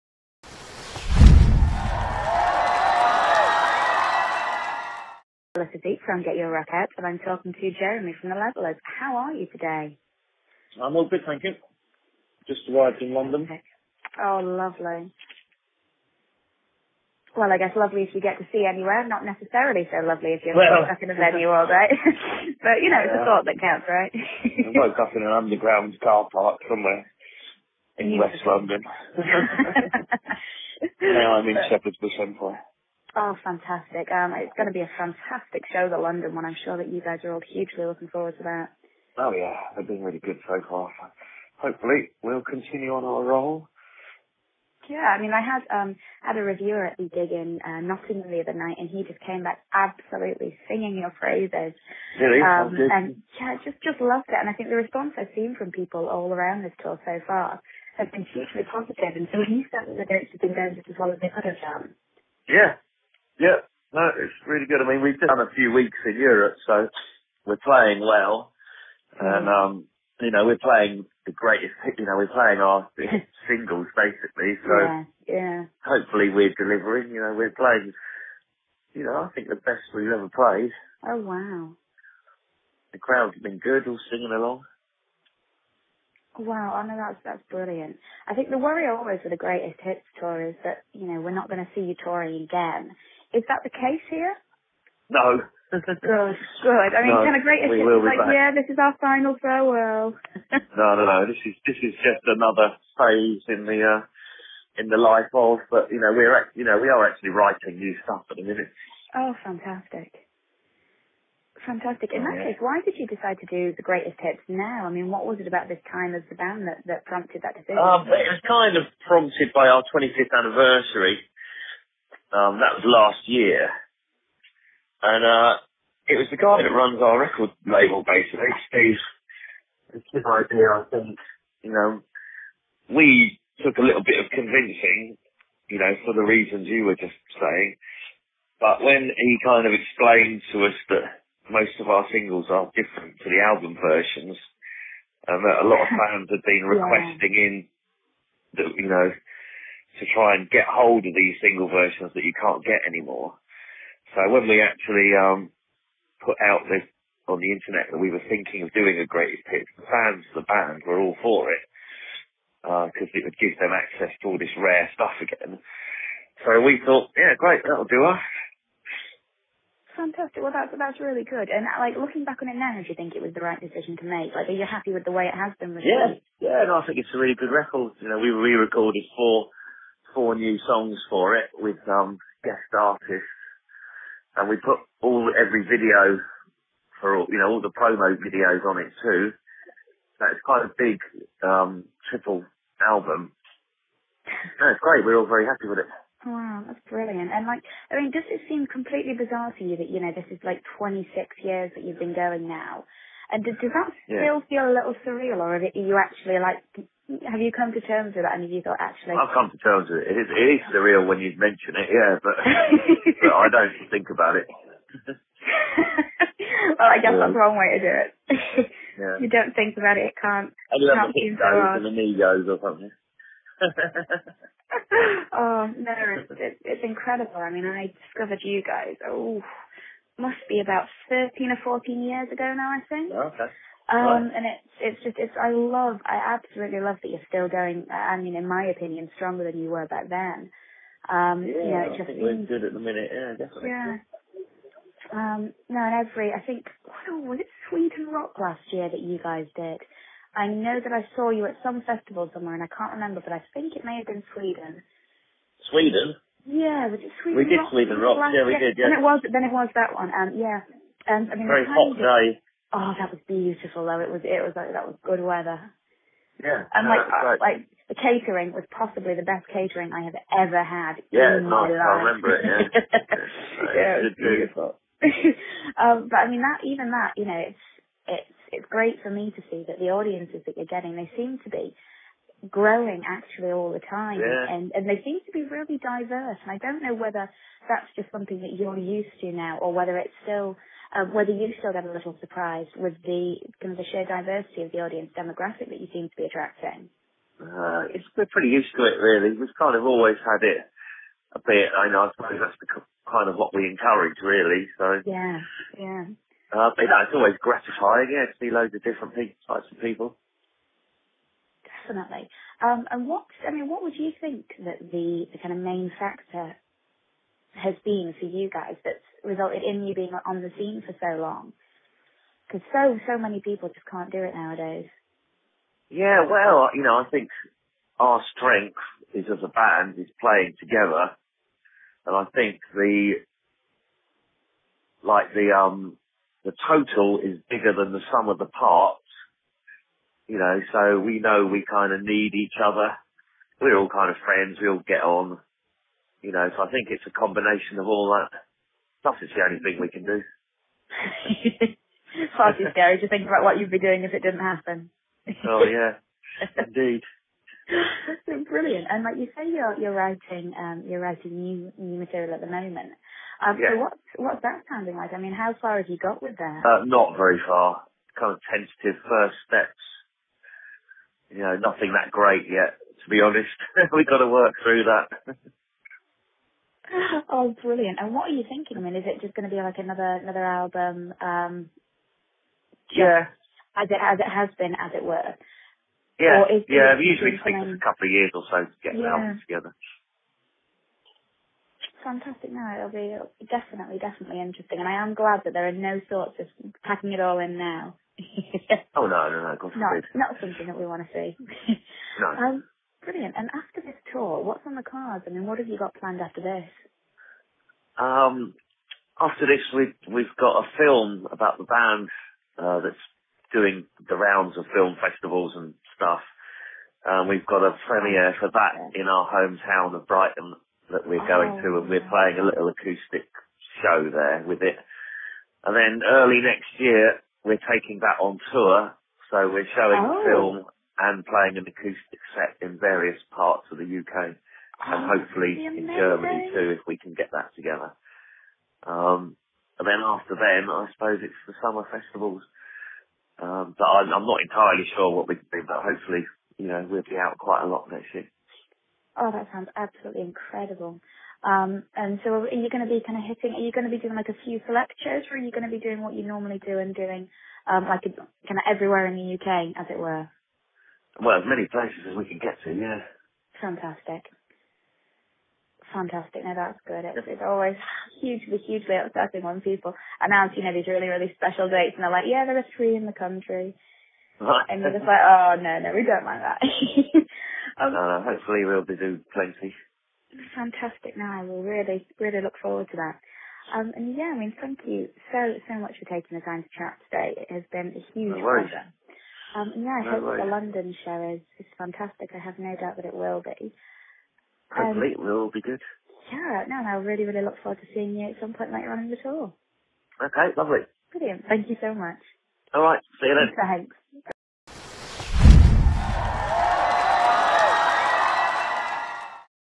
The Levellers interview
GetYourRockOut chatted to The Levellers on their latest UK tour. For more interviews, news and reviews, head over